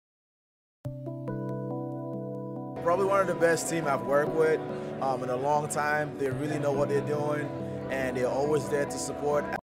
Biogen's Journey with Riskonnect | Excellent Customer Support
Today we're shining a light on Biogen's journey with Riskonnect.
"They're one of the best teams I've worked with -- in a long time," says John Mbengam.